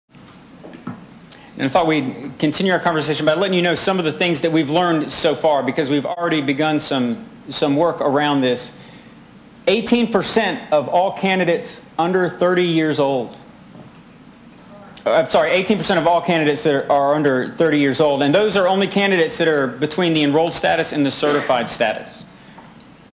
The Rev. Trip Lowery talks about the Young Clergy Initiative - Spring 2013
Eighteen percent of candidates for ordained ministry in the UMC are under age 30,  the Rev. Trip Lowery, GBHEM's director of Young Adult Ministry Discernment and Enlistment.